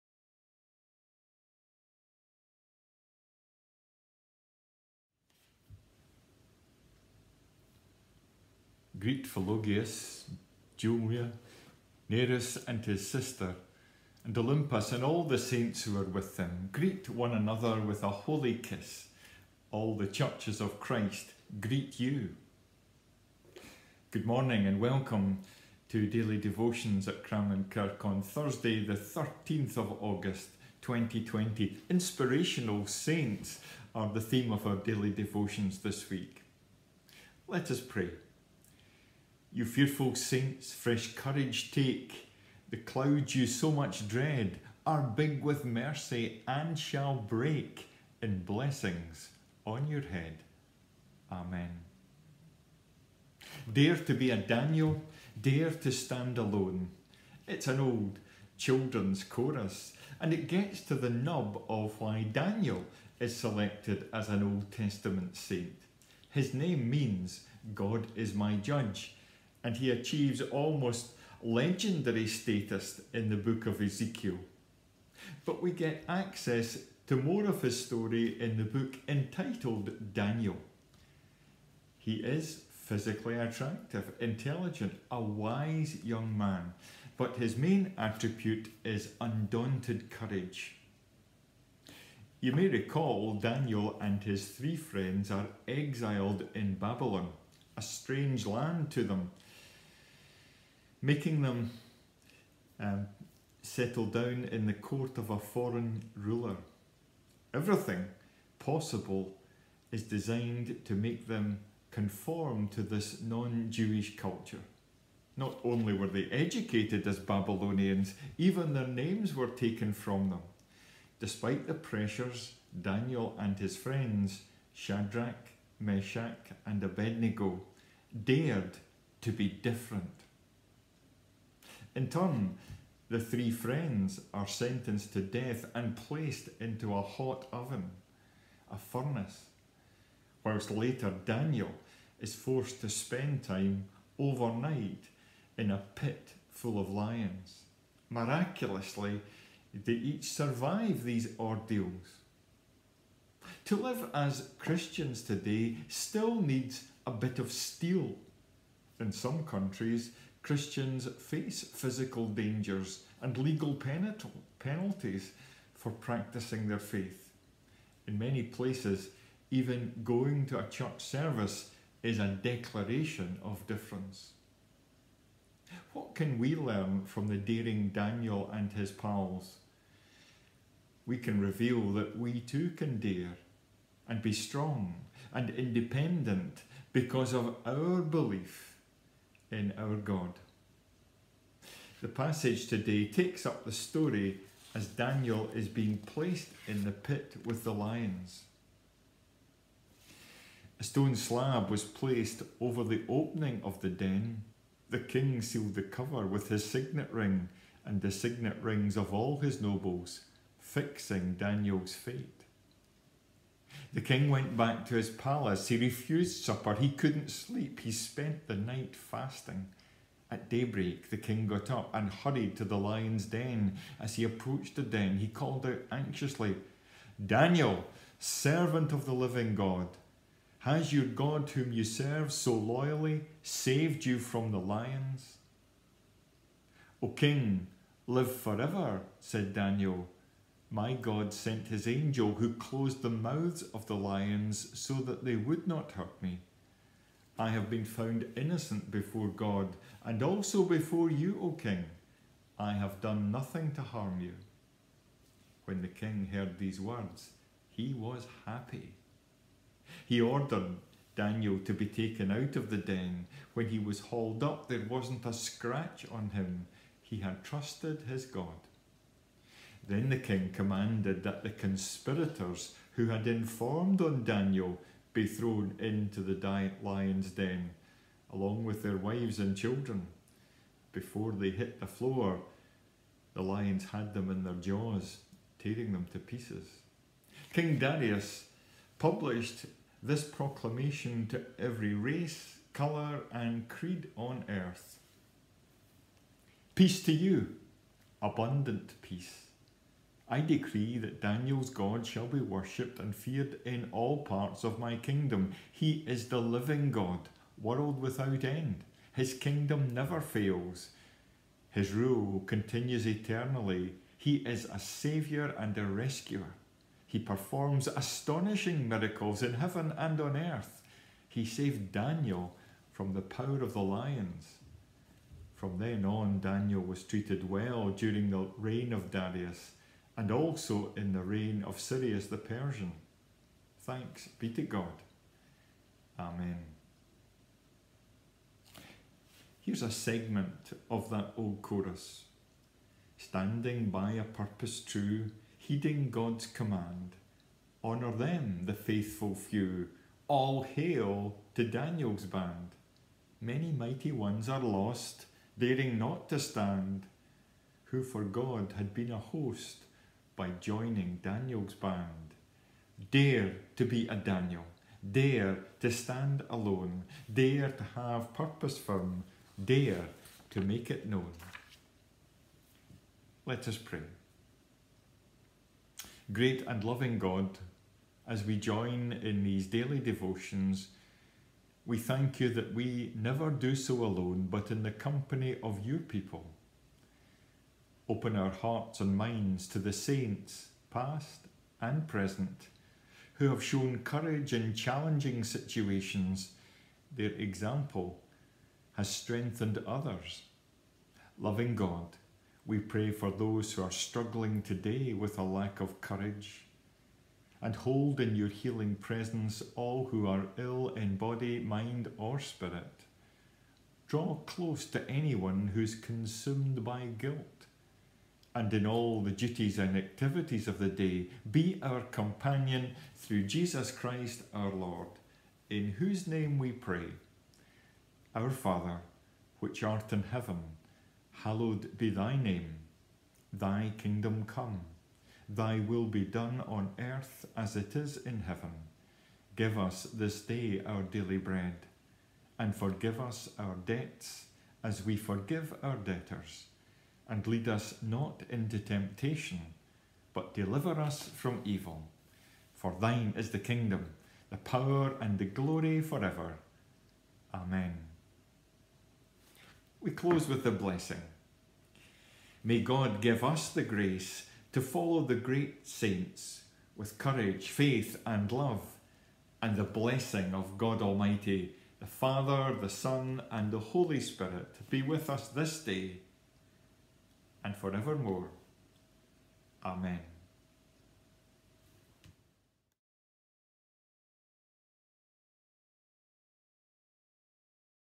Daily Devotions 13 August
A short service of daily devotions with Ian Gilmour of Cramond Kirk